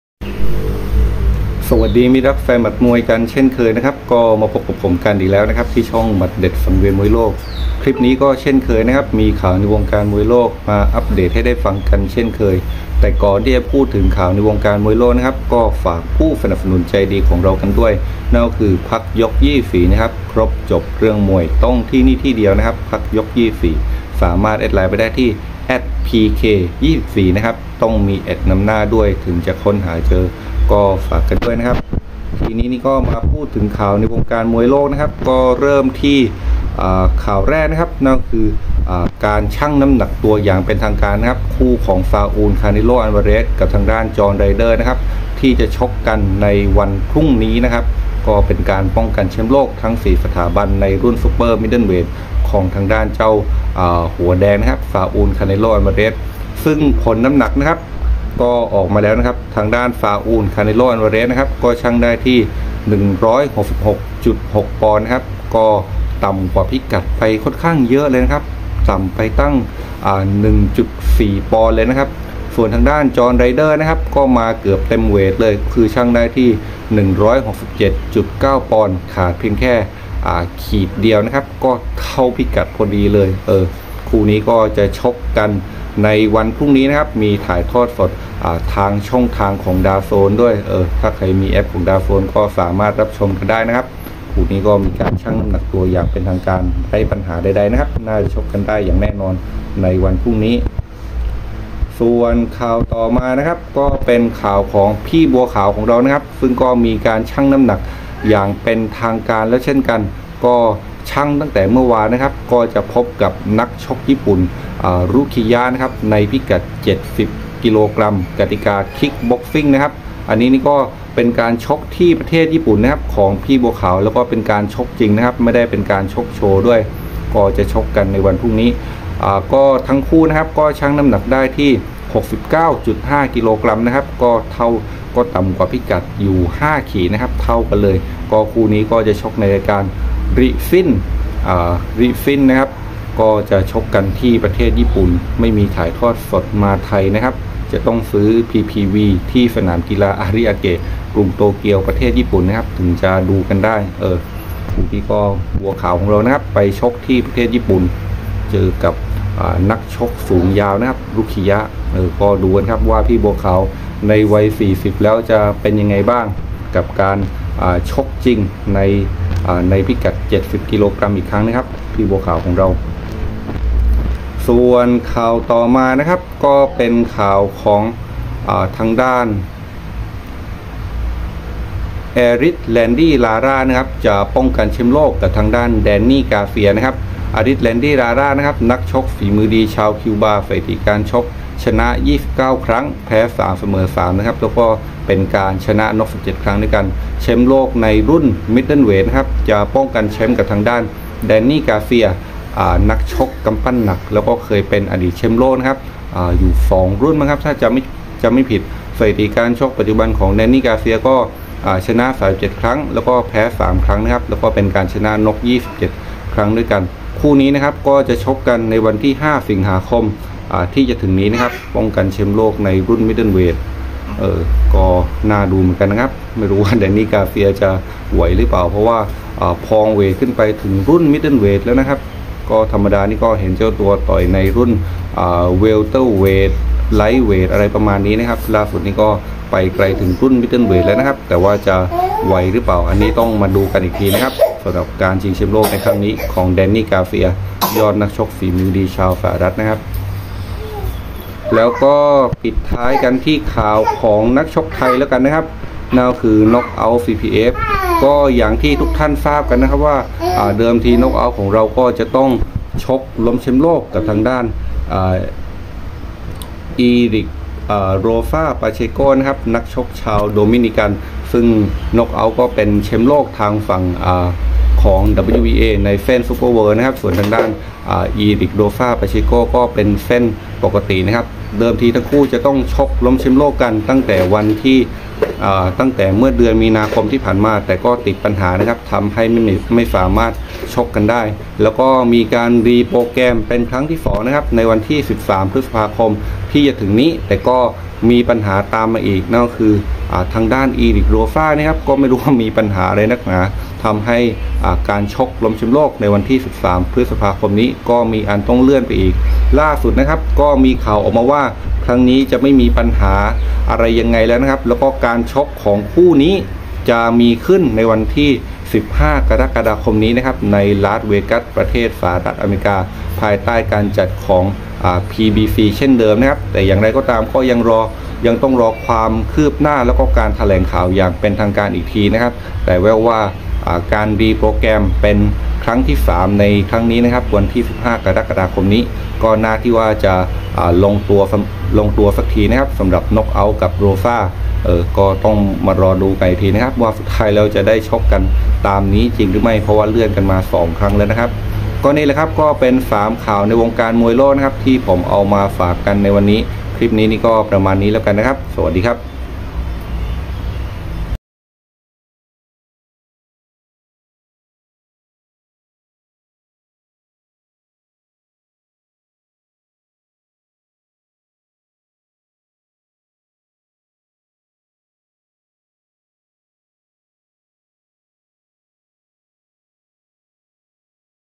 ข่าวมวยโลกล่าสุด(6/5/2566)
1.คาเนโล่ น้ำหนักผ่านพร้อมดวลไรเดอร์พรุ่งนี้
2.บัวขาวชกวันนี้ที่ญี่ปุ่น
3.เเดนนี่ การ์เซีย ชิงเเชมป์โลกรุ่นมิดเดิ้ลเวท
4.น็อคเอาท์vsโรซ่า 15ก.ค.นี้

ทางไลน์ tubtib33